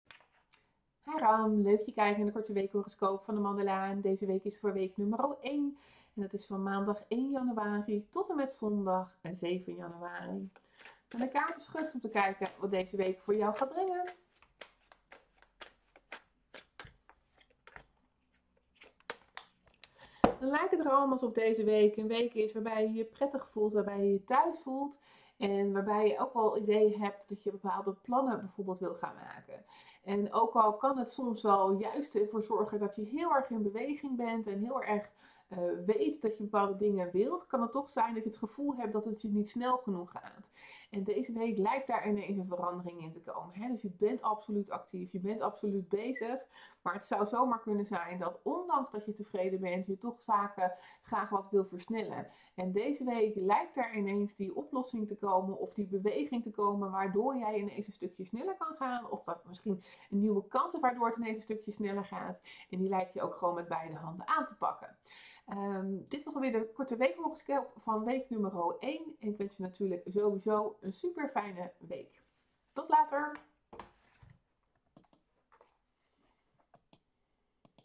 Weekhoroscoop Ram week 1 2024
Korte tarot weekhoroscoop Ram week 1 2024 - maandag 1 januari t/m zondag 7 januari